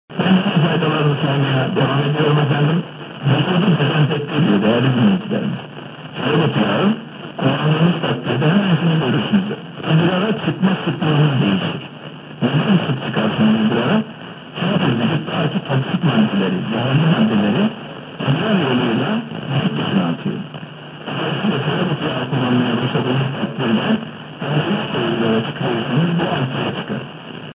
[ Tropo ] 95.8 MHz - pres. Bayram FM - Istanbul/Camlica - Turkey - 1044 km
Strong tropospheric propagation.
10 October 2013, 02:07 UTC. QTH: Poltava, Ukraine.
Equipment: Sony XDR-F1HD + 5 el. Yagi (vertical).